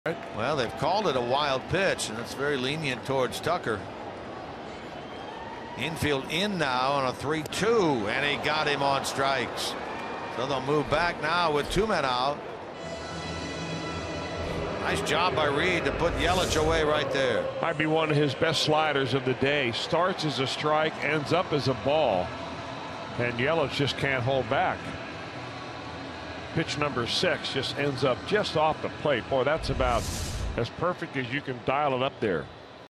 CIN@MIA: Reed strikes out his seventh of the game
Cody Reed gets Christian Yelich to strike out swinging in the bottom of the 5th inning